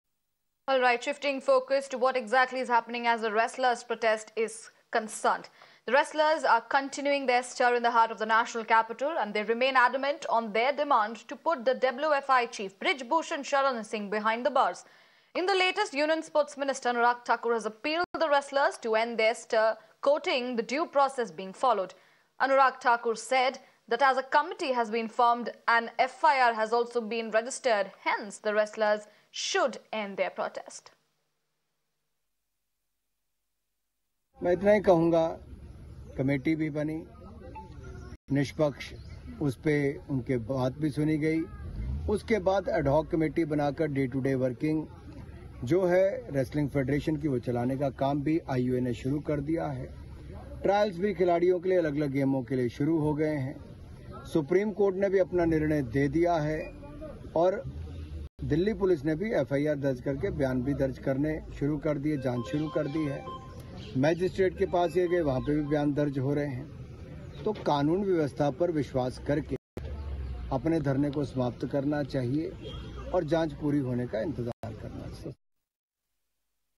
Union Minister Anurag Thakur Urges Wrestlers To End Protest Says 'Have Faith In Law & Order'
As per the latest news, the wrestlers are continuing their stir in the National Capital's heart and remain adamant on their demand to put the WFI chief Brij Bhushan Sharan Singh behind bars. In the latest, Union Sports Minister Anurag
Thakur has appealed to the wrestlers to end their stir, quoting the due process being followed.
"The committee was formed, and their concerns were also heard. An ad-hoc committee has been formed to start the day-to-day work of the Wrestling Federation by the Indian Olympic Association (IOA). Trials have also started for athletes. Supreme Court has given its judgment. Delhi Police has also registered an FIR and is recording statements. The magistrate is also registering statements. They must have faith in our law and order and end their protest.", says Anurag Thakur. Watch the complete video now to know more.